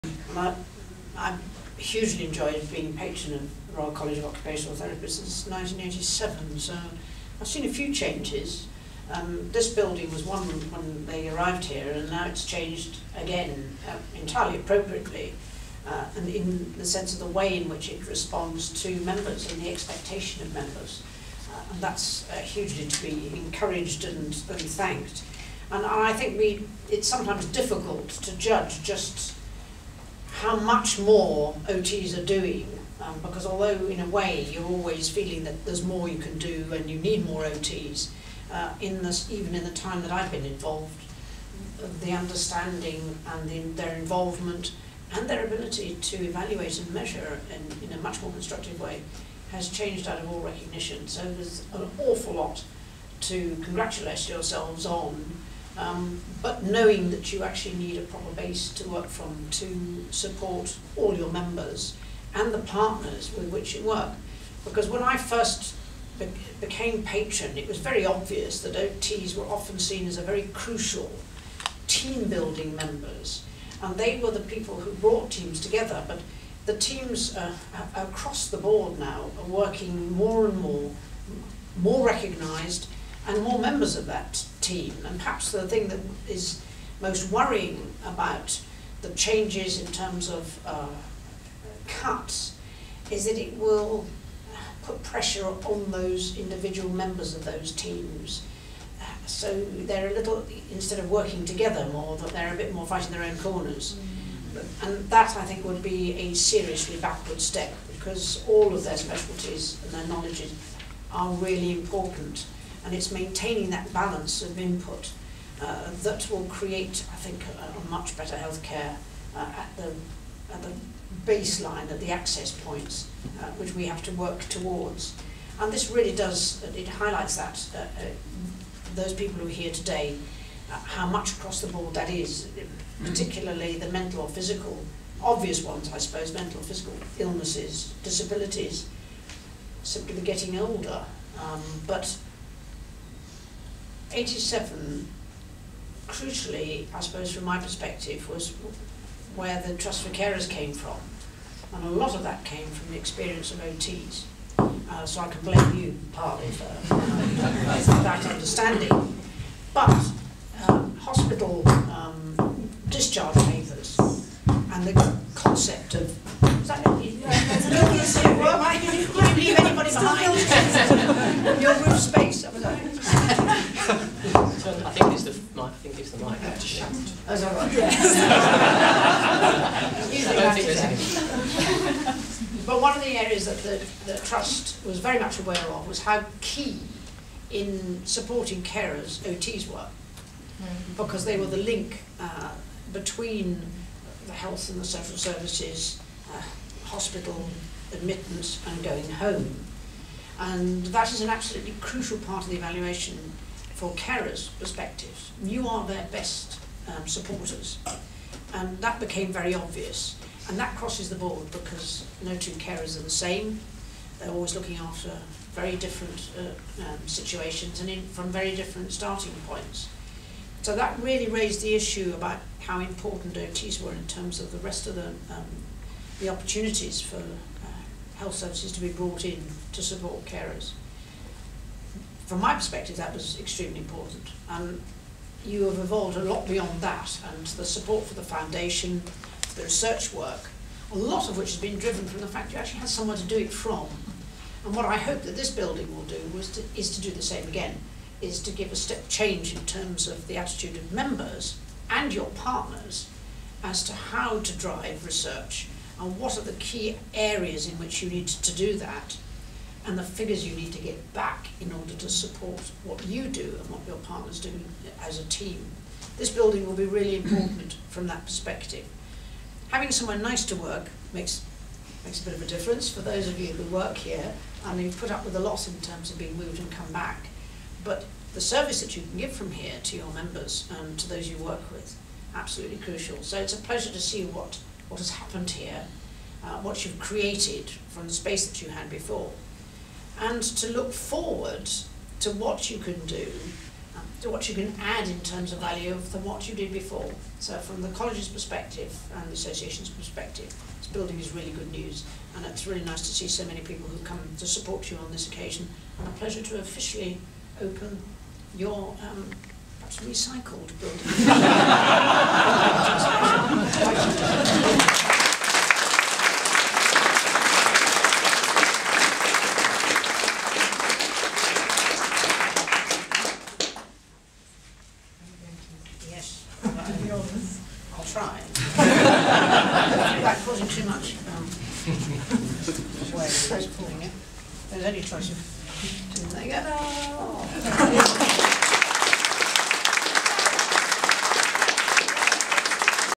HRH Princess Royal speaking at the re-opening of the College of Occupational Therapists
Princess Anne, Patron of the College of Occupational Therapists, speaks about the importance of occupational therapy at the re-opening of the College of Occupational Therapists on Borough High Street on 5 May 2011.